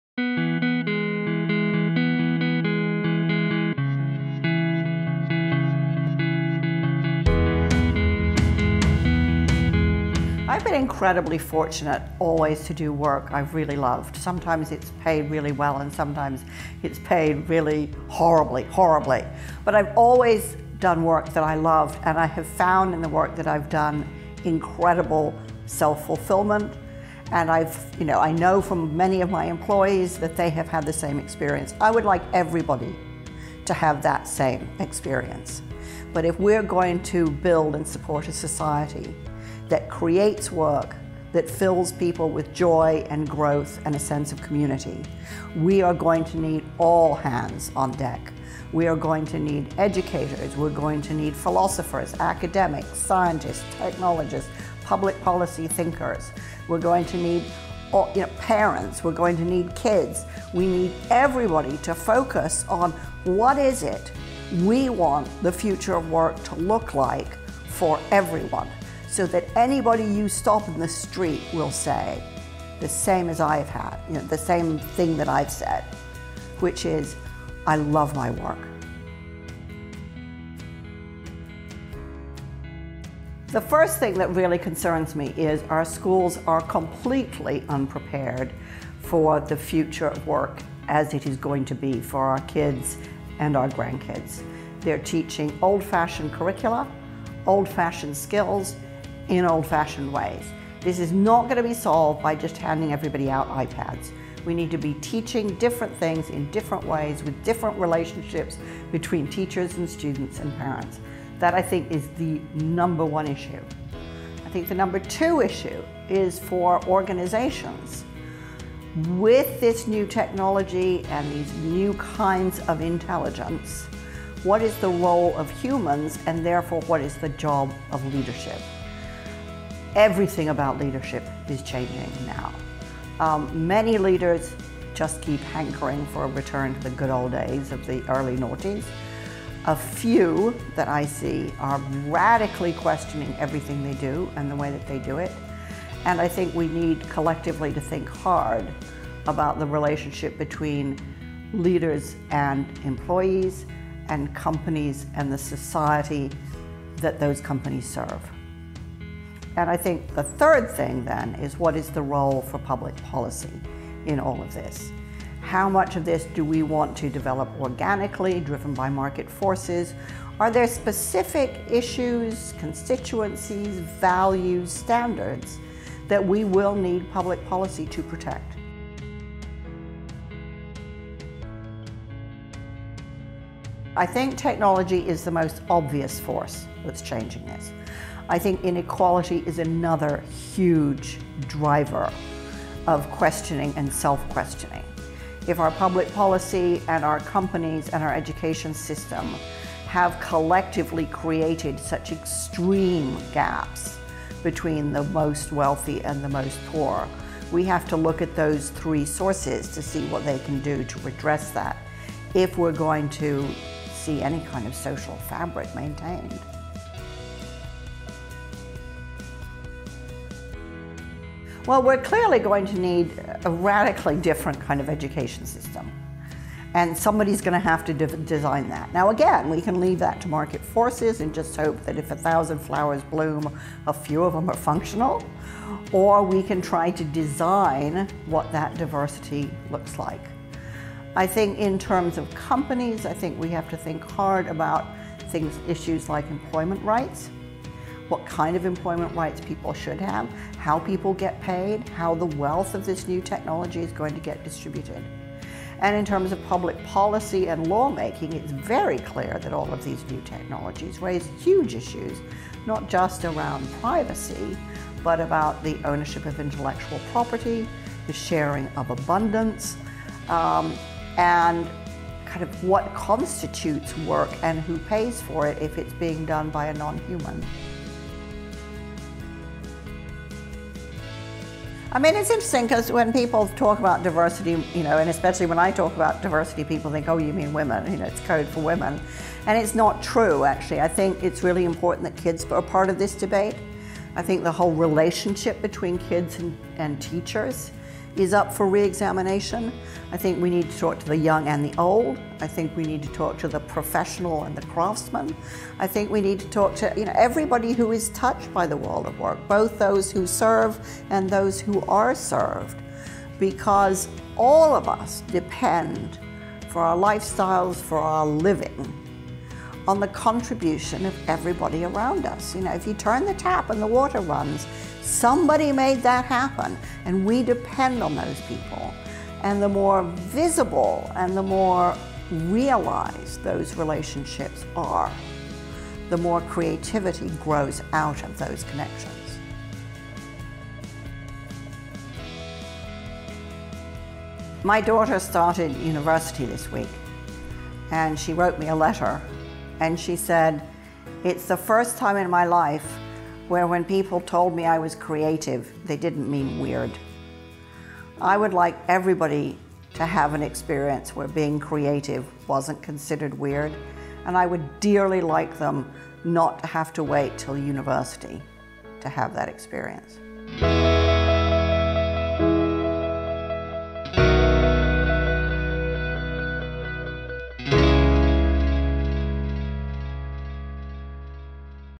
Margaret Heffernan: The human future of work needs our involvement (Full Interview)
Margaret Heffernan, author of Willful Blindness and A Bigger Prize, talks about the need for everyone to be involved in the debate about the human future of work. She calls out the key issues and actions and explains what diversity really means for work, life and creativity.